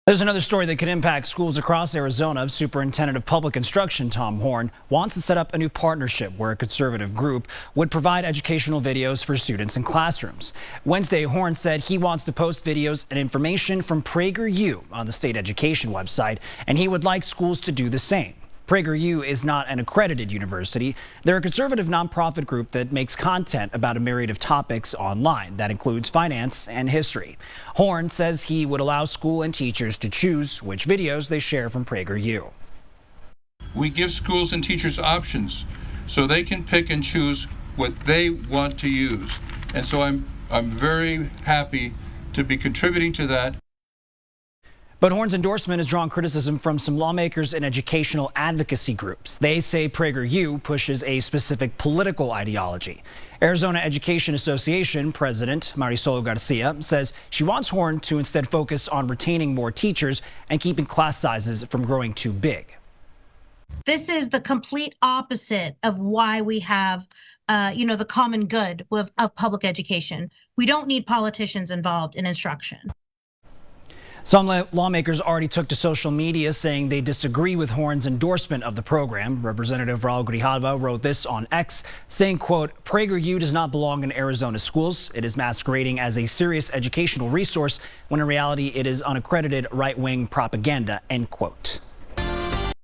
Controversial curriculum proposal in AZ schools
Use of curriculum from PragerU has drawn criticism as 'right wing propaganda'.